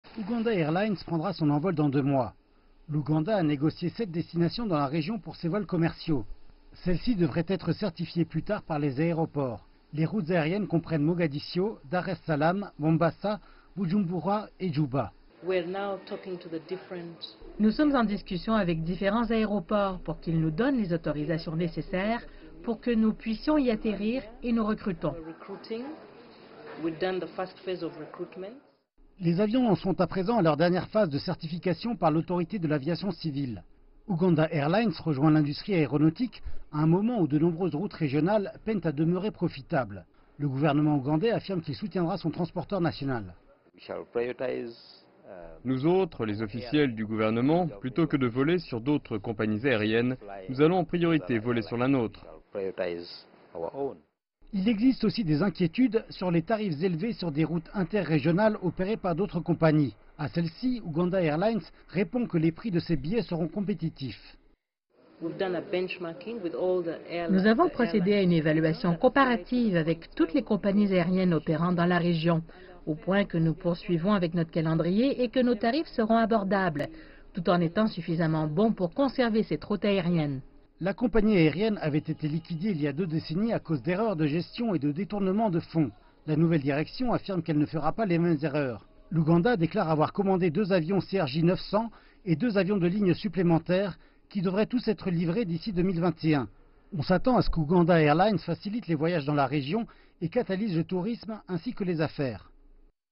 La compagnie aérienne nationale est prête pour ses 1er vols commerciaux après un hiatus de 17 ans
L'Ouganda a déclaré que sa compagnie aérienne nationale était prête à effectuer ses premiers vols commerciaux, après avoir sécurisé ses routes dans la région. Deux avions de passagers ont été commandés au constructeur canadien Bombardier, et sont arrivés dans le pays en avril. Ces livraisons font partie des efforts du pays pour ressusciter Uganda Airlines, après 17 ans sans transporteur national.